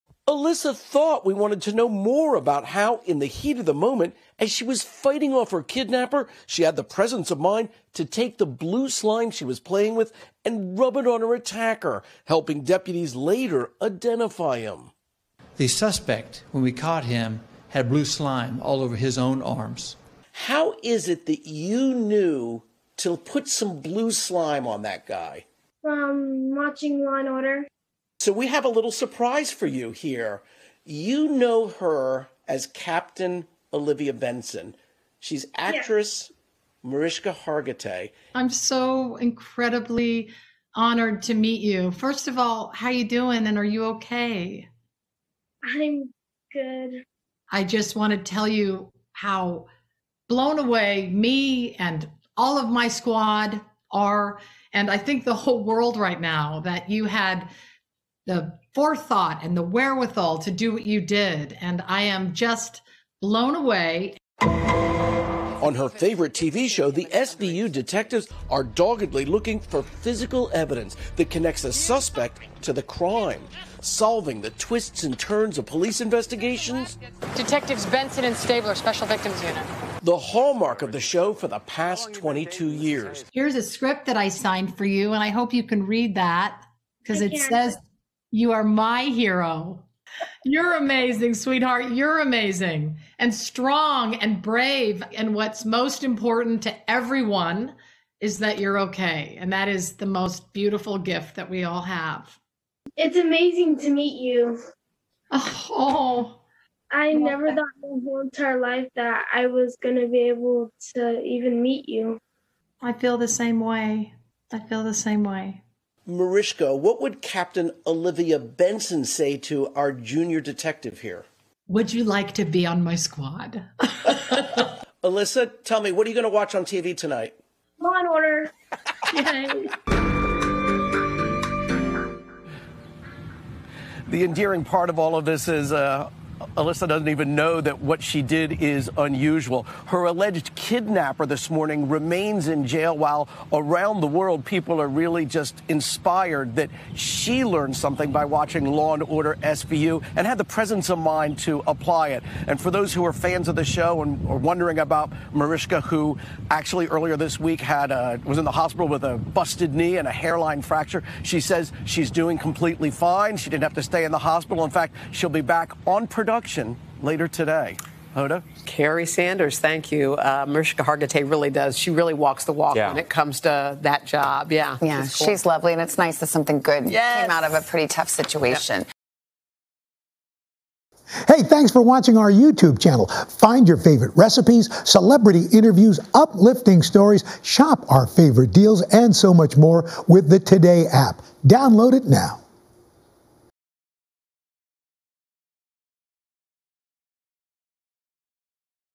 Mariska Hargitay of ‘SVU’ Surprises Girl Who Fought Off Attacker
An 11-year-old girl in Florida used knowledge gained watching “Law & Order: Special Victims Unit” to fight off an attacker and help police catch him. She got a surprise visit from “SVU” star Mariska Hargitay.